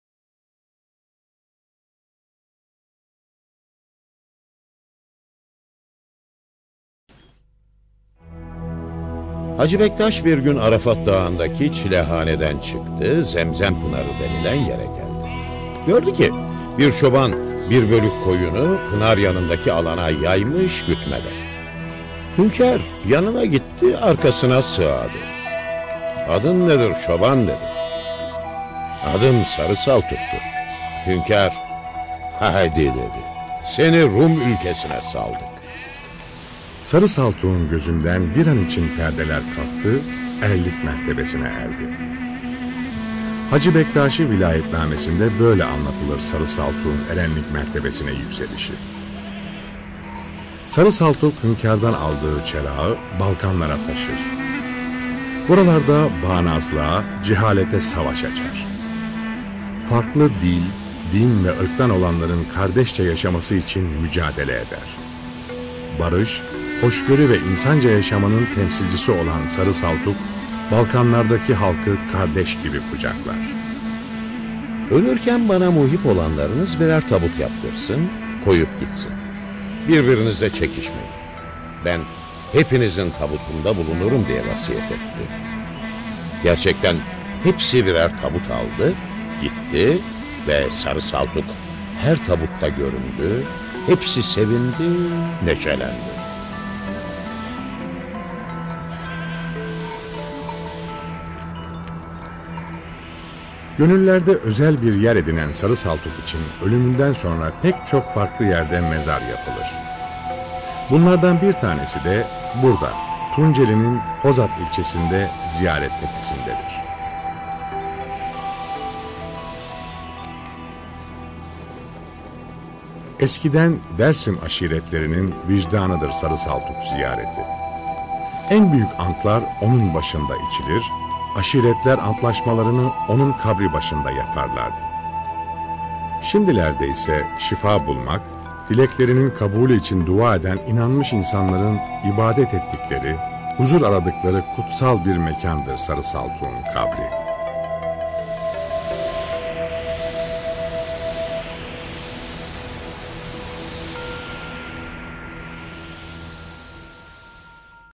ANADOLU ERENLERİ: SARI SALTUK- Belgesel
Anadolu Erenlerinden Sarı Saltuk hakkında; hikayesi, yaşamı, dergahı vb bilgi içerir